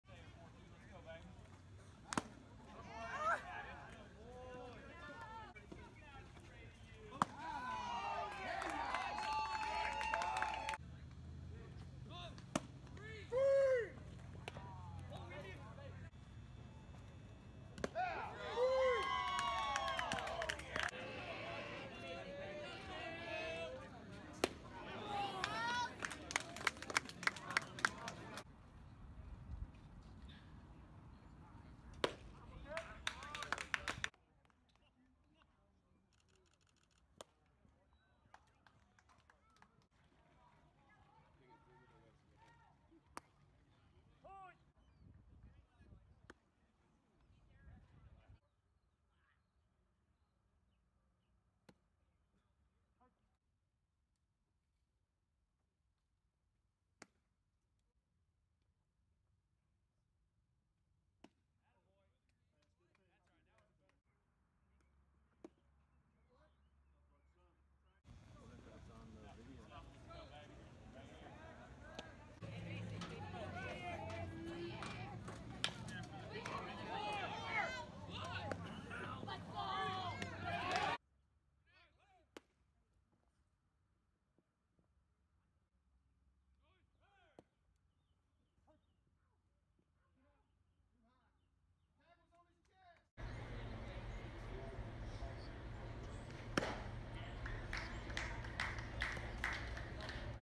Justin Breitenstein - Baseball Highlights (Pitching) - Fall 2020
Fall 2020 Pitching Highlights from RHP Justin Breitenstein, Cannons Baseball Academy. Class of 2023, Estero (Fla.) High School.